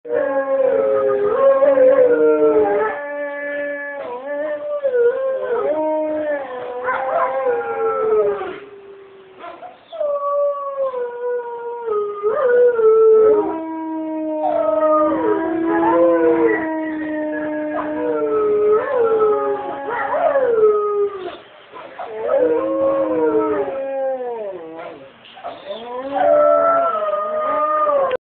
Howling wolves @ The Folsom Zoo.
The wolves @ The Folsom Zoo were howling like crazy that morning.